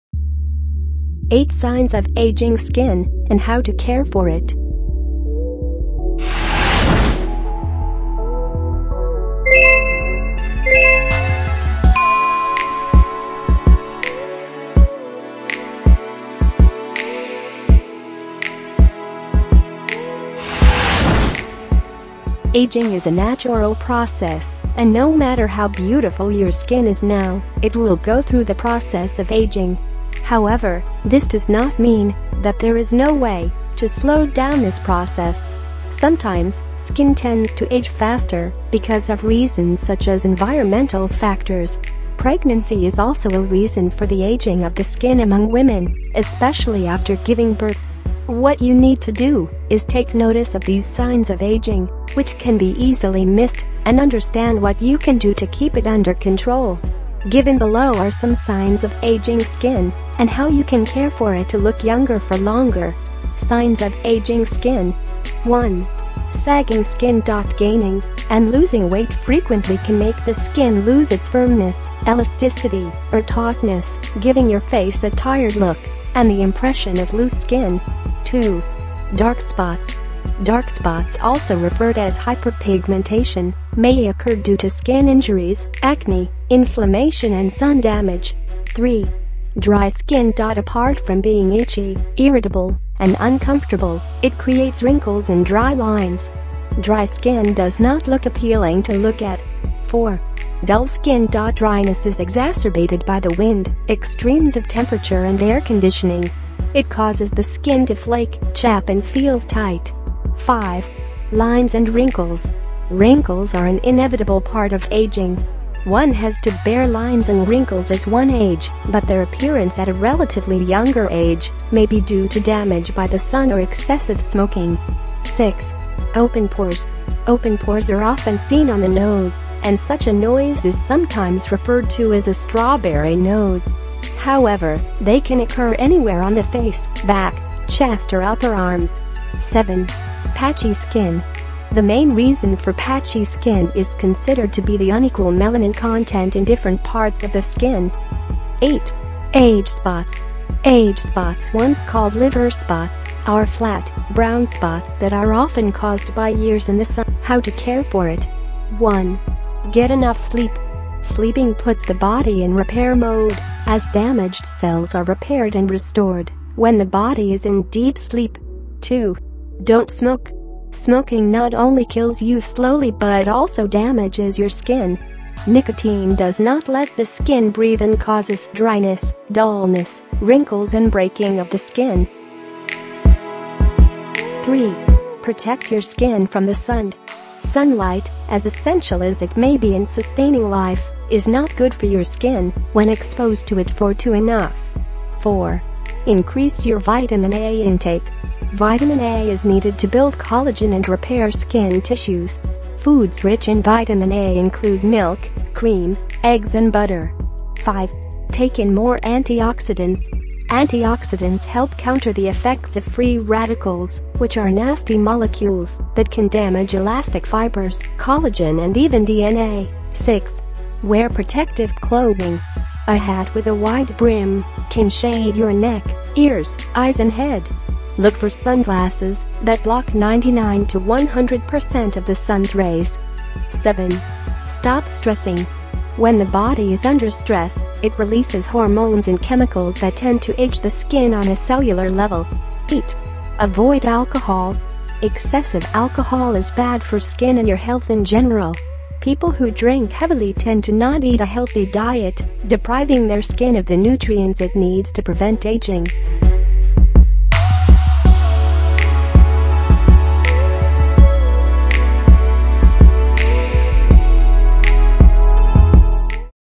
8 Signs Of Ageing Skin And How To Care For It
8 Signs Of Ageing Skin And How To Care For It.

Ageing is a natural process and no matter how beautiful your skin is now, it will go through the process of ageing. However, this does not mean that there is no way to slow down this process. Sometimes, skin tends to age faster because of reasons such as environmental factors. Pregnancy is also a reason for the ageing of the skin among women, especially after giving birth. What you need to do is take notice of these signs of ageing, which can be easily missed, and understand what you can do to keep it under control.

Given below are some signs of ageing skin and how you can care for it to look younger for longer.
Signs of ageing skin.

1. Sagging skin .

Gaining and losing weight frequently can make the skin lose its firmness, elasticity or tautness, giving your face a tired look and the impression of loose skin.

2. Dark spots.

Dark spots also referred as hyperpigmentation, may occur due to skin injuries, acne, inflammation and sun damage.

3. Dry skin .

Apart from being itchy, irritable and uncomfortable, it creates wrinkles and dry lines. Dry skin does not look appealing to look at.

4. Dull skin .

Dryness is exacerbated by the wind, extremes of temperature and air-conditioning. It causes the skin to flake, chap and feels tight.

5. Lines and wrinkles.

Wrinkles are an inevitable part of ageing. One has to bear lines and wrinkles as one age, but their appearance at a relatively younger age may be due to damage by the sun or excessive smoking.

6. Open pores.

Open pores are often seen on the nose, and such a noise is sometimes referred to as a "strawberry nose". However, they can occur anywhere on the face, back, chest or upper arms.

7. Patchy skin.

The main reason for patchy skin is considered to be the unequal melanin content in different parts of the skin. Other reasons may include the growth of foreign organisms on the skin and vascular changes.

8. Age spots.

Age spots once called “liver spots,” are flat, brown spots that are often caused by years in the sun. They are bigger than freckles and commonly show up on areas of the face, hands, arms, back and feet.

How to care for it.

1. Get enough sleep.

Sleeping puts the body in repair mode, as damaged cells are repaired and restored when the body is in deep sleep.

2. Don’t smoke.

Smoking not only kills you slowly but also damages your skin. Nicotine does not let the skin breathe and causes dryness, dullness, wrinkles and breaking of the skin. It also ruins the skin’s elasticity.

3. Protect your skin from the sun.

Sunlight, as essential as it may be in sustaining life, is not good for your skin when exposed to it for too enough. Wear a broad-spectrum sunscreen, with an SPF of at least 15, that shields you from both UVA and UVB rays.

4. Increase your Vitamin A intake.

Vitamin A is needed to build collagen and repair skin tissues. Foods rich in Vitamin A include milk, cream, eggs and butter.

5. Take in more antioxidants.

Antioxidants help counter the effects of free radicals, which are nasty molecules that can damage elastic fibres, collagen and even DNA. Tomato, tea, berries, red grapes and garlic are all dietary sources of antioxidants.

6. Wear protective clothing.

A hat with a wide brim can shade your neck, ears, eyes and head. Look for sunglasses that block 99 to 100 percent of the sun’s rays. If you have to be in the sun, wear loose, lightweight, long-sleeved shirts and long pants or long skirts.

7. Stop stressing.

When the body is under stress, it releases hormones and chemicals that tend to age the skin on a cellular level. Try yoga, meditation or other activities are known to release stress.

8. Avoid alcohol

Excessive alcohol is bad for skin and your health in general. People who drink heavily tend to not eat a healthy diet, depriving their skin of the nutrients it needs to prevent ageing.